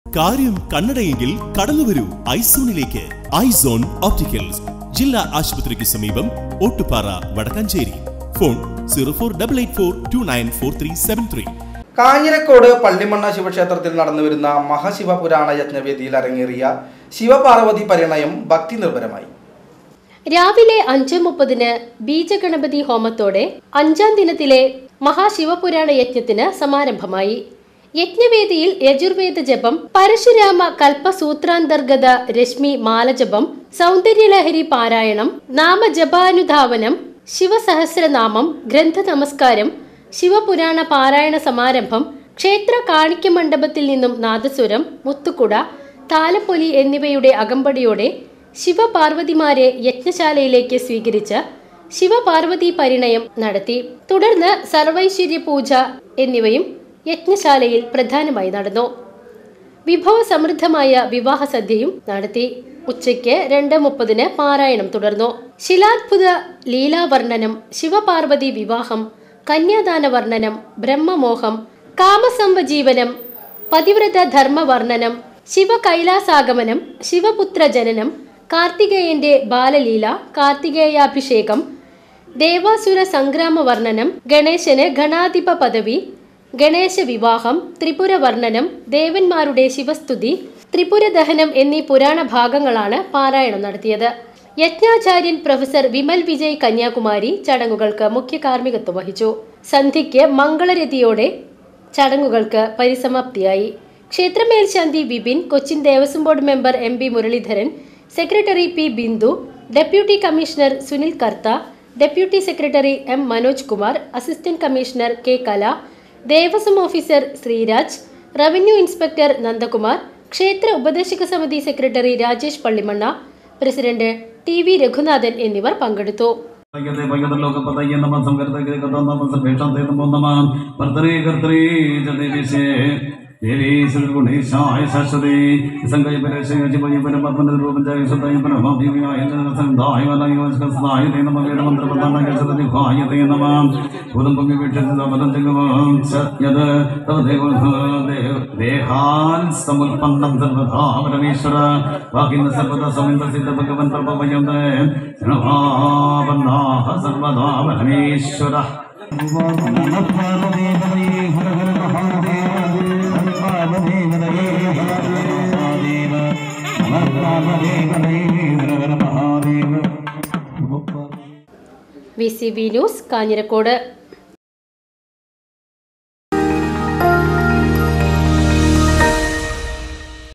കാഞ്ഞിരക്കോട് പള്ളിമണ്ണ ശിവക്ഷേത്രത്തിൽ നടന്നുവരുന്ന യജ്ഞ വേദിയിൽ ശിവ പാർവ്വതി പരിണയം അരങ്ങേറി
കാഞ്ഞിരക്കോട് പള്ളിമണ്ണ ശിവക്ഷേത്രത്തിൽ നടന്നുവരുന്ന മഹാ ശിവ പുരാണ യജ്ഞ വേദിയിൽ ശിവ പാർവ്വതി പരിണയം അരങ്ങേറി

VCV NEWS

VCV NEWS CHANNEL
Wadakkanchery, Thrissur